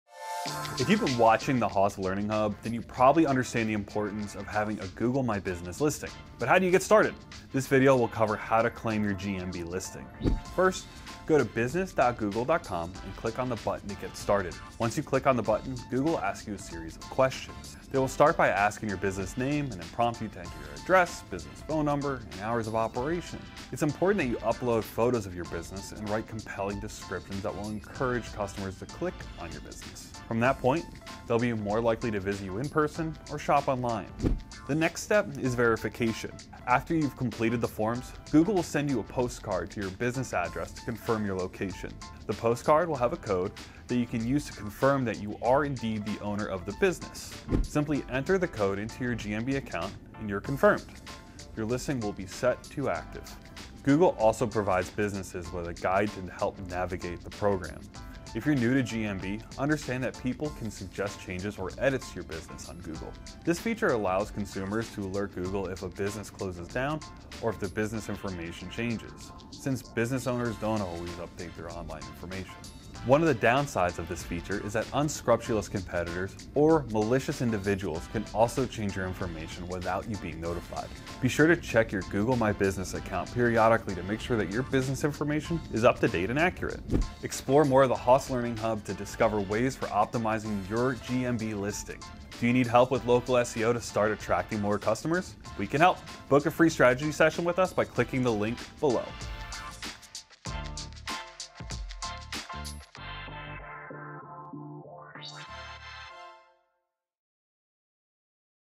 How to Claim Your Google Business Profile (GBP) Listing
Welcome back for Episode 28 of The Hoth's Learning Hub Series. These videos will help you better understand SEO and help you become an SEO expert fast.

Today we're discussing how to claim your Google Business Profile (GBP) Listing - also known as your Google My Business (GMB) Listing.

Be sure to check out our free SEO guides that follow along with our videos: